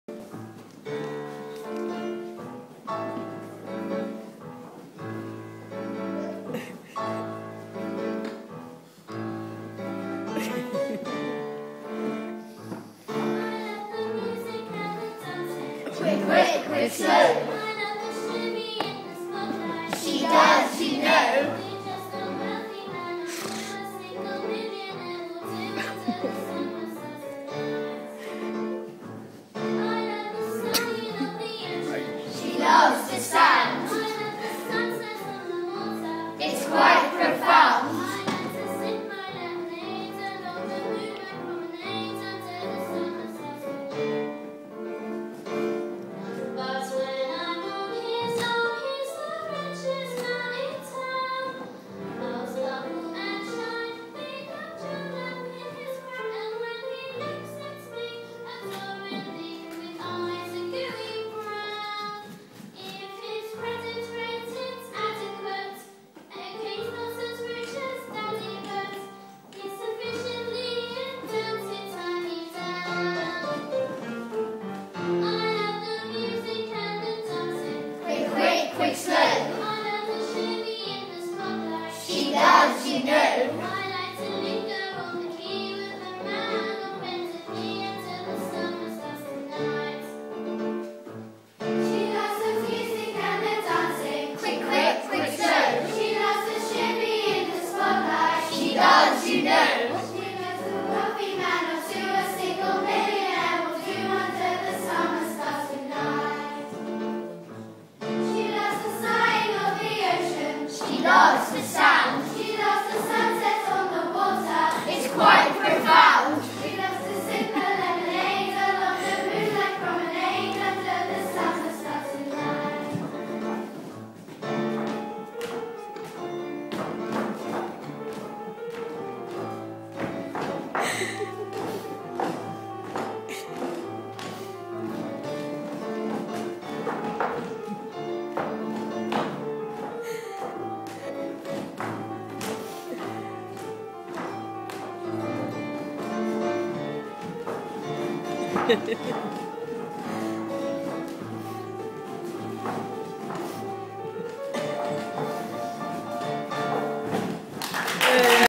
Oil shaw at Aldrington
My son couldn't stop the giggles in his end of term play